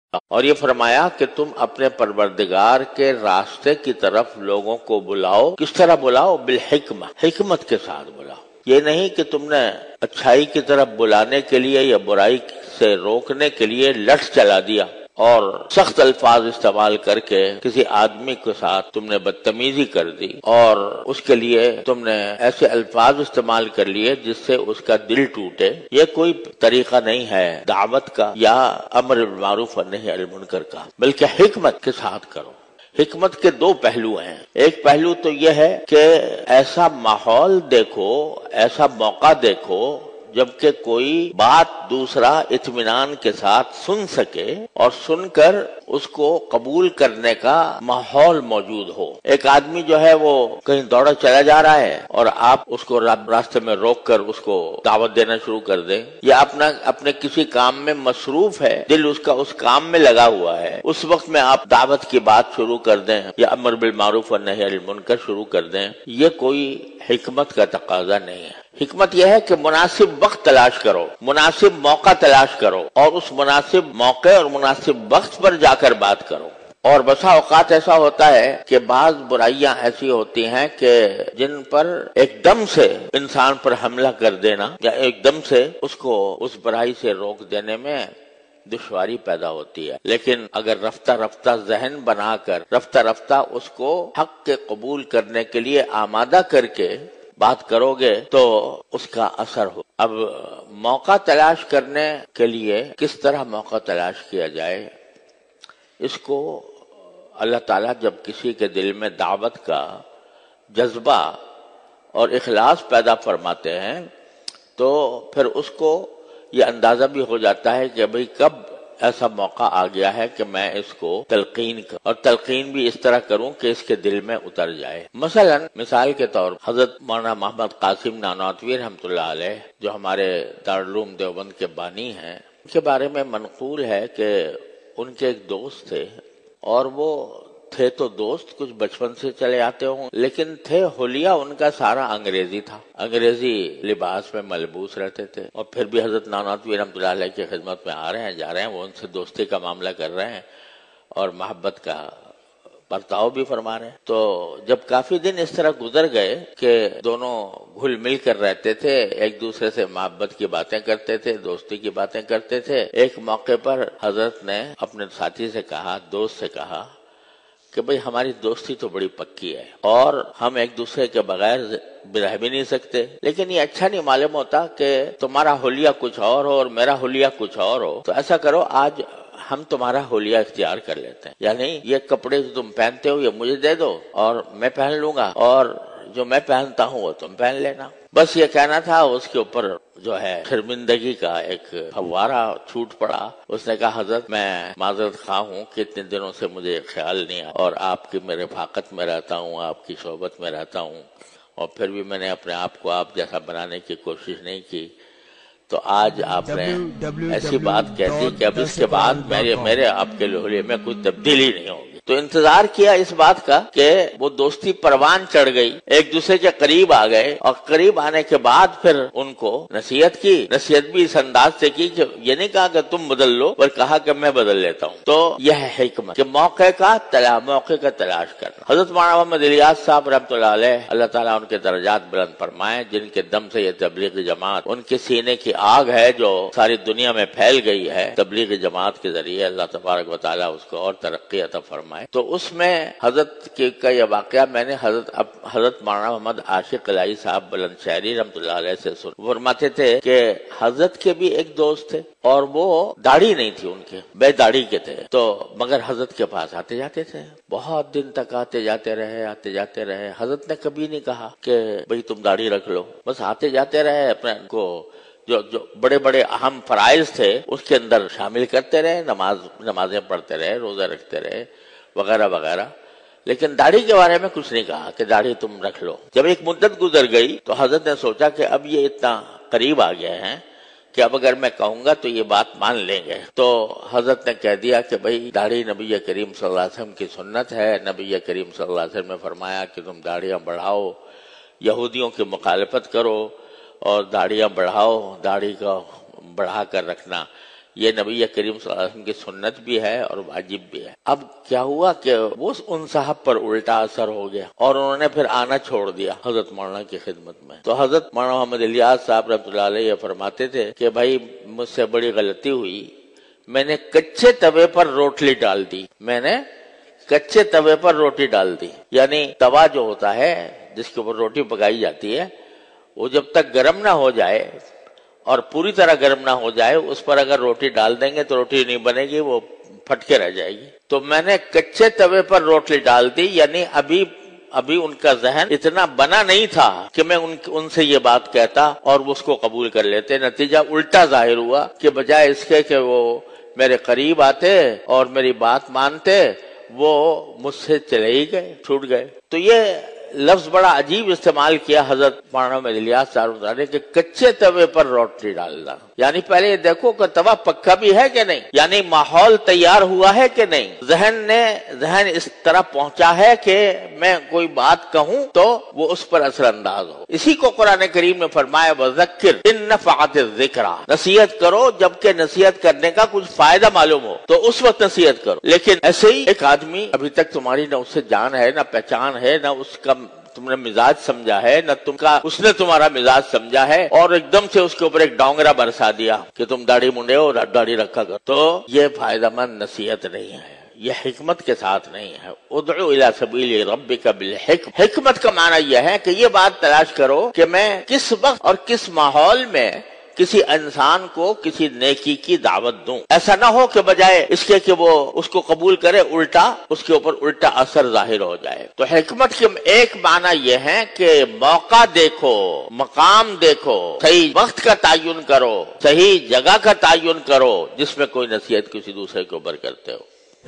Deen Ki Dawat Main Is Qurani Usool Par Amal Karain Mufti Muhammad Taqi Usmani Sahab دین کی دعوت میں
Deen Ki Dawat Main Is Qurani Usool Par Amal Karain Mufti Muhammad Taqi Usmani Sahab

 دین کی دعوت میں اس قرآنی اصول پر عمل کریں